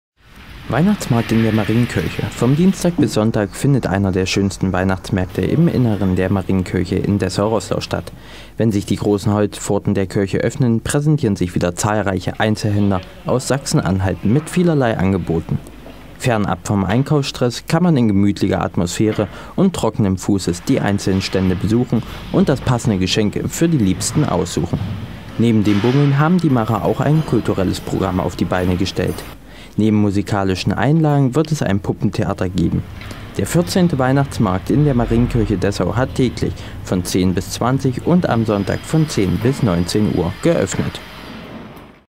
Weihnachtsmarkt in Marienkirche - RAN1
Weihnachtsmarkt in Marienkirche
Vom Dienstag bis Sonntag findet der einer der schönsten Weihnachtsmärkte im inneren der Marienkirche in Dessau-Roßlau statt. Wenn sich die großen Holzpforten der Kirche offnen präsentieren sich wieder zahlreiche Einzelhändler  aus Sachsen-Anhalt mit vielerlei Angeboten. Fernab vom Einkaufsstress kann man in gemütlicher Atmosphäre und trockenen Fußes die einzelnen Stände besuchen und das passende Geschenk für die liebsten aussuchen. Neben dem Bummeln haben die Macher auch ein kulturelles Programm auf die Beine gestellt. Neben musikalischen Einlagen wird es ein Puppentheater geben. Der 14. Weihnachtsmarkt in der Marienkirche Dessau hat täglich von 10 bis 20 Uhr und am Sonntag von 10 bis 19 Uhr geöffnet.